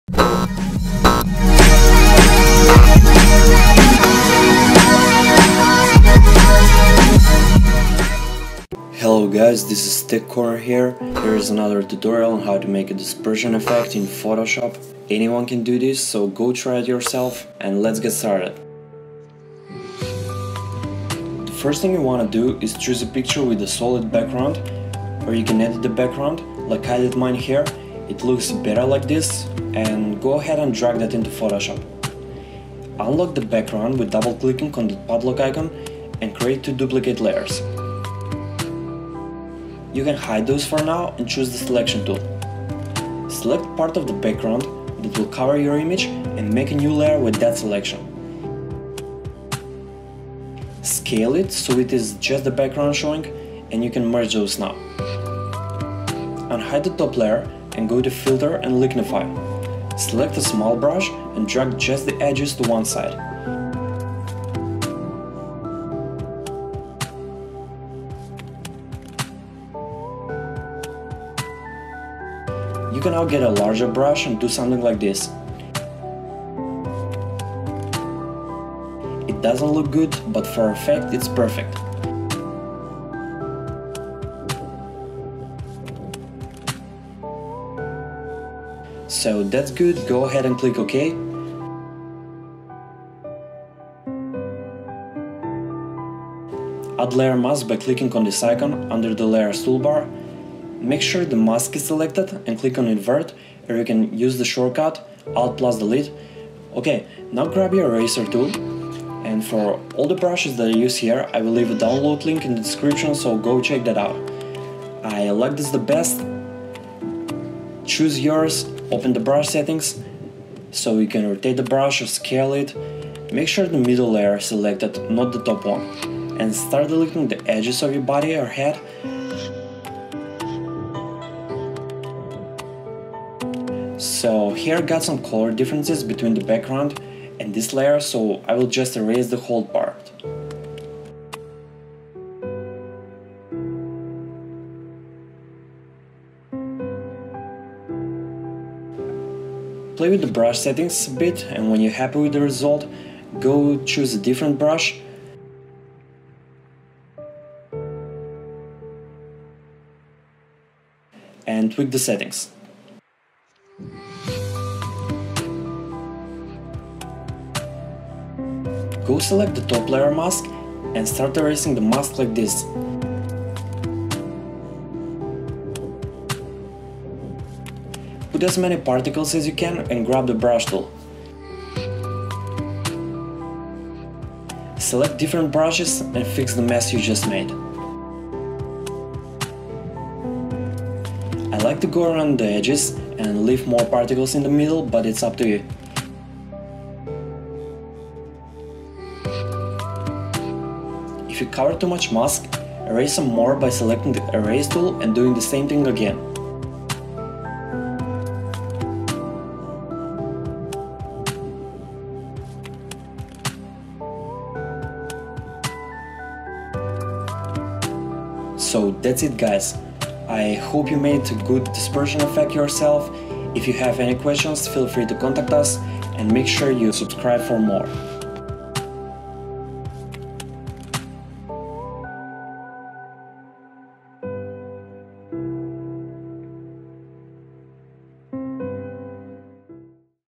Dispersion Effect Photoshop Fast
Easy and fast tutorial on how to make your own dispersion effect using Photoshop in less than 5 minutes!!